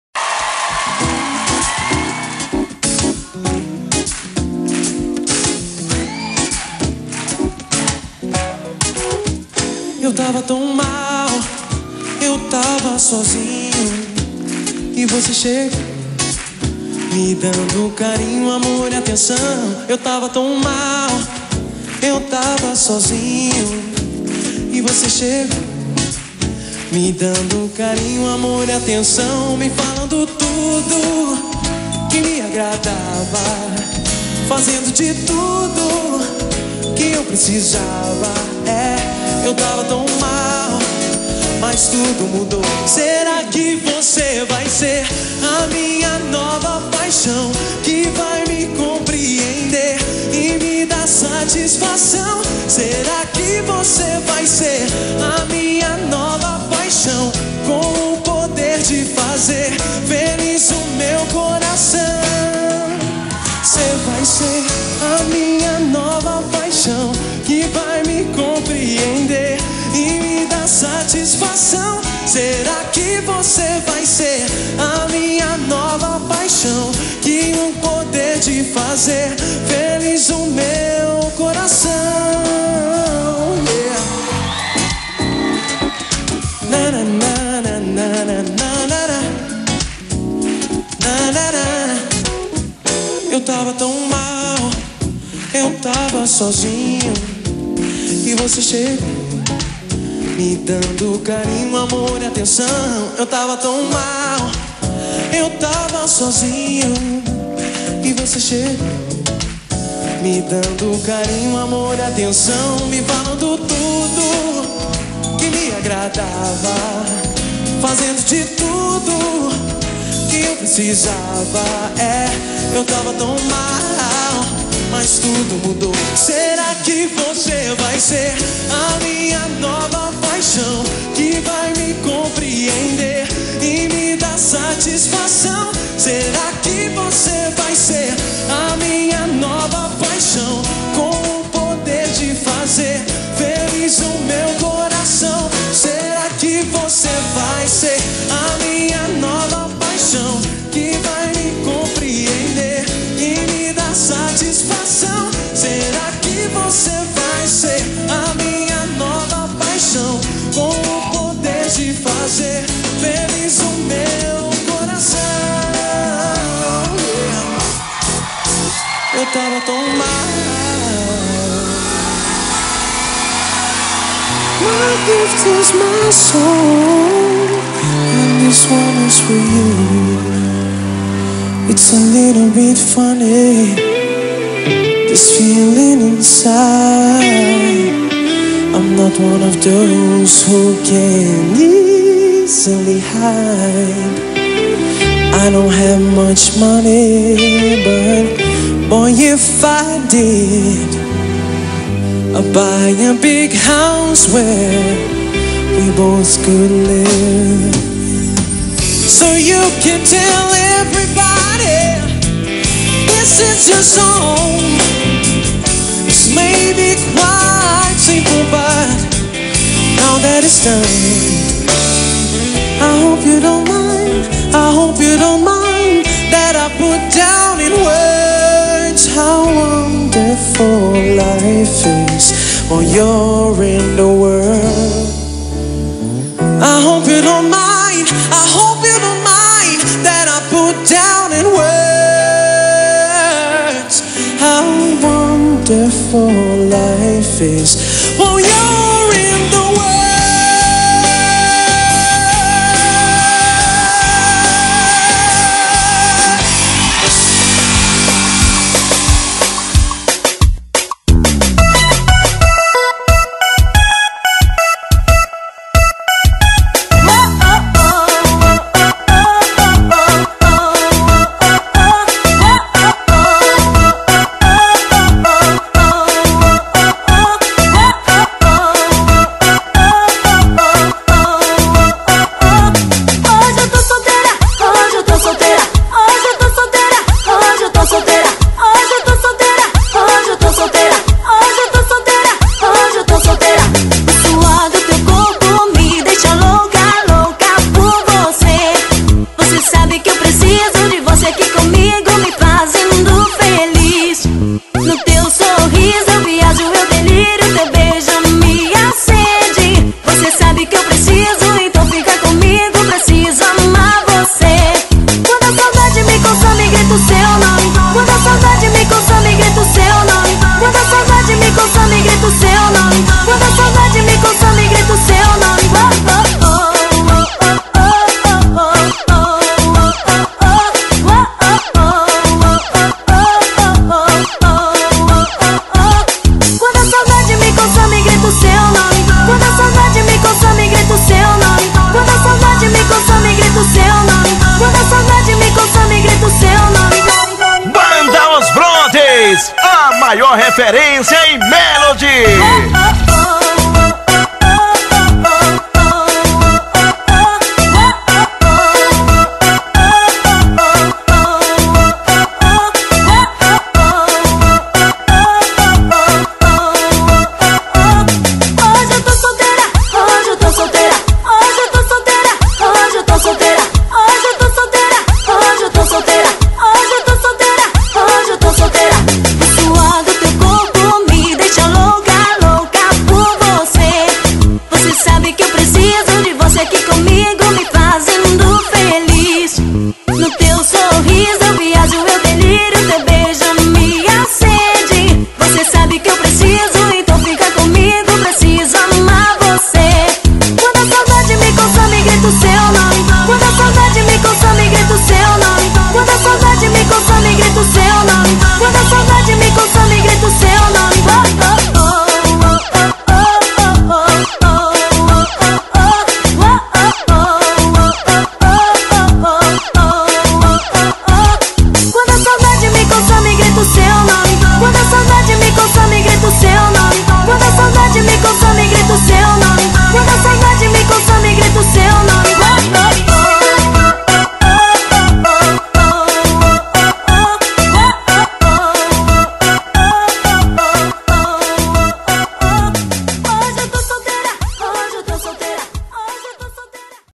Saulo Roston Homenagem do Fco De Belém
*Fco SauloRoston-PA Sempre Com Você* Fez Uma Linda Homenagem ao Grande Idolo Do Brasil  *Saulo Roston*...